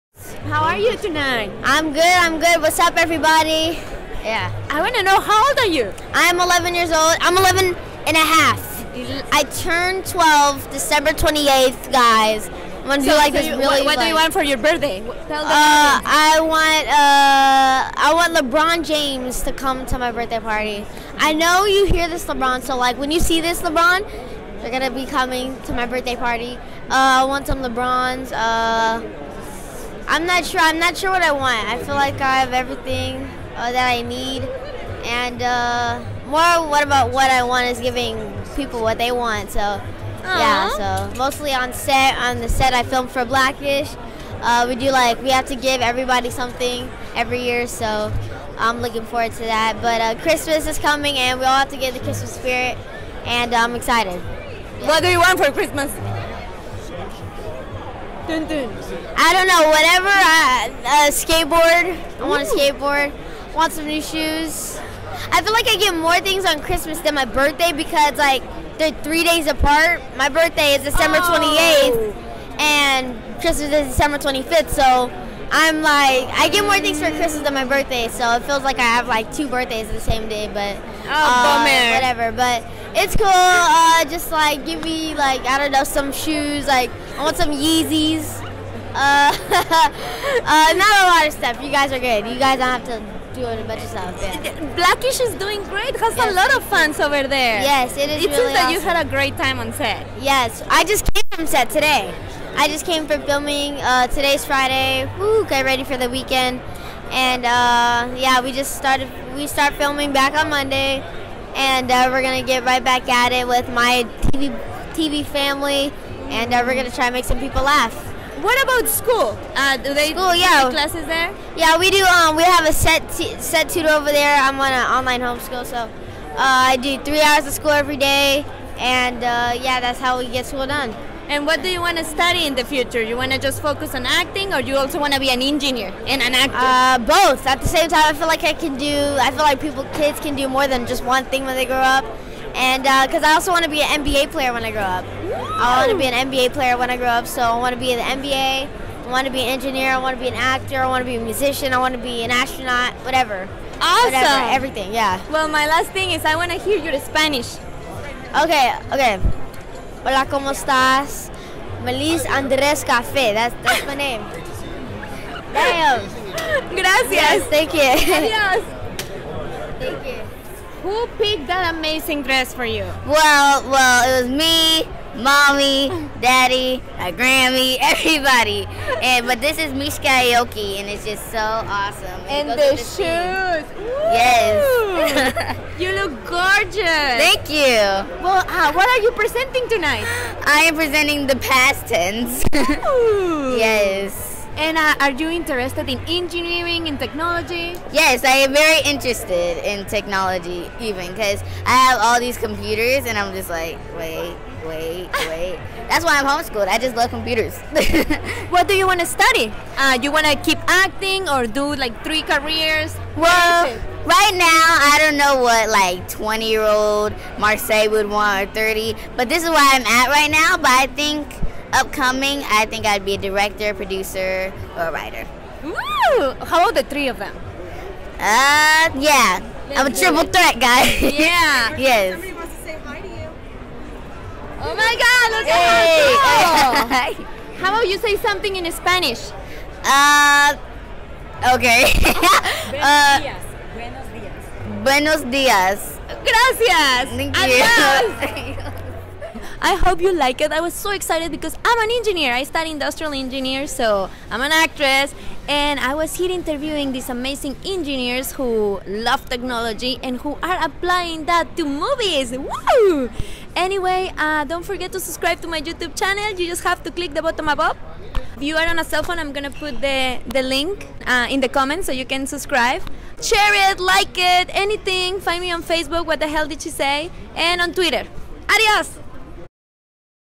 Fun interview at SMPTE with Miles Brown and Marsai Martin from Black-ish
The two youngest presenters at the SMPTE Centennial Gala  Miles Brown and Marsai Martin from Black-ish talked to the Pink iPhone for What the hell did she say?